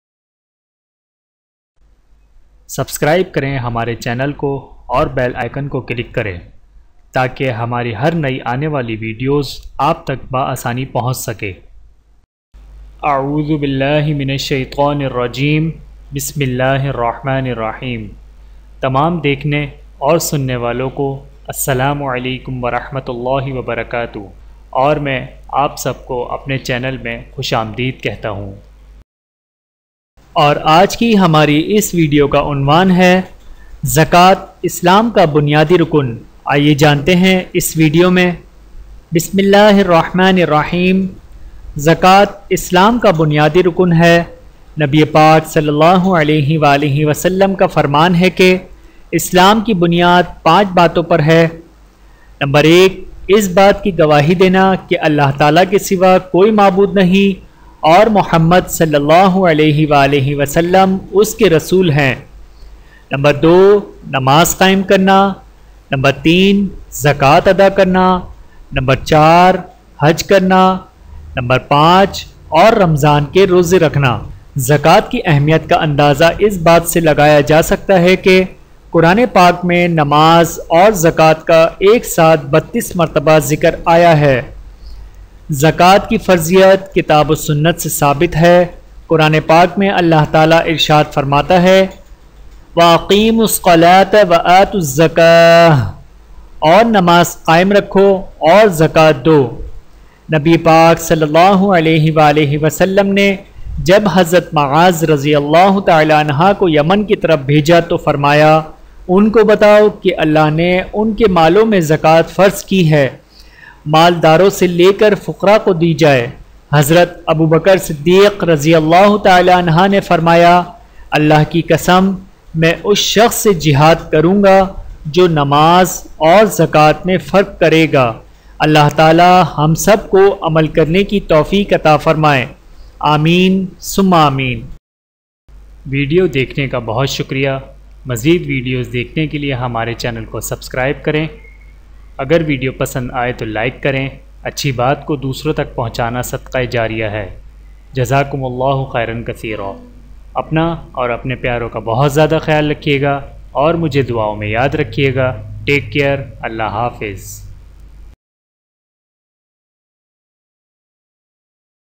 Zakat in Islam | Zakat islam Ka Aham Rukan Hai | زکوٰۃ | زکوٰۃ اسلام کا اہم رکن ہے | زکوۃ کے مسائل
1 - || " Aao Quran Seekhain or Sikhaye " ||

زکوٰۃ  اسلام کا بنیادی رکن ہے نبی پاک ﷺ  کا فرمان ہے کہ اسلام کی بنیاد پانچ باتوں پر ہے.

1۔ اس بات کی گواہی دینا کہ اللہ تعالٰی کے سوا کوئی معبود نہیں اور محمد ﷺ اس کے رسول ہیں۔

2۔ نماز قائم کرنا۔

3۔ زکوٰۃ ادا کرنا۔

4۔ حج کرنا۔

5۔ اور رمضان کے روزے رکھنا۔

زکوٰۃ کی اہمیت کا اندازہ اس بات سے لگایا جا سکتا ہے کہ قرآن پاک میں نماز اور                                                                                        زکوٰۃ کا ایک ساتھ 32 مرتبہ ذکر آیا ہے۔

زکوٰۃکی فرضیت کتاب و سنت سے ثابت ہے۔ قرآن پاک میں اللہ تعالٰی ارشاد فرماتا ہے

وَاَقِیْمُوا الصَّلٰوۃَ وَاٰتُوا الزَّ   کٰوۃَ

اور نماز قائم رکھو اور زکوٰۃ دو۔

نبی پاک ﷺ نے جب حضرت معاذ رضی اللہ عنہ کو یمن کی طرف بھیجا تو فرمایا:
ان کو بتاؤ کہ اللہ نے ان کے مالوں میں زکوٰۃ فرض کی ہے، مال داروں سے لے کر فقراء کو دی جائے۔

حضرت ابوبکر صدیق رضی اللہ عنہ نے فرمایا اللہ کی قسم !
میں اس شخص سے جہاد کروں گا جو نماز اور زکوٰۃ میں فرق کرے گا۔

اللہ ہم سب کو عمل کرنے کی توفیق عطا فرمائیں
آمین

شکریہ